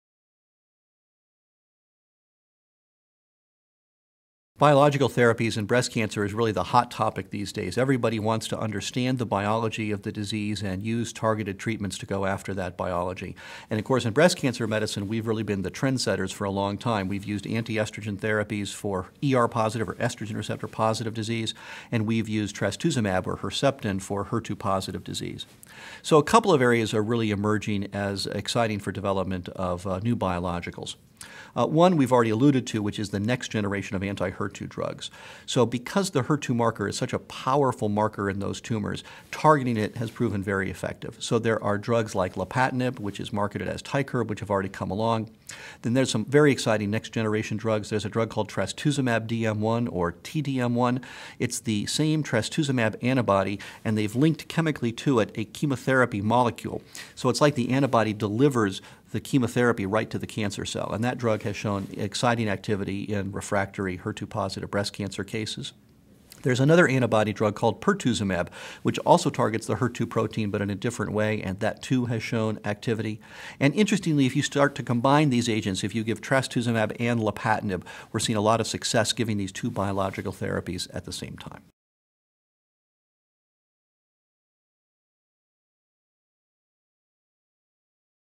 Cancer expert explains targeted (biological) therapy for breast cancer
Find out why biological or targeted therapies are a "hot topic" in breast cancer. Harold Burstein, MD, PhD, of the Dana-Farber Cancer Institute, an expert in treating breast cancer, talks about the newest cancer-fighting drugs in this video from the National Comprehensive Cancer Network (NCCN). The NCCN is a not-for-profit alliance of 21 leading cancer centers—including Dana-Farber Cancer Institute—dedicated to improving care for cancer patients. The NCCN Guidelines for Patients detail these and other treatments for breast and other cancers.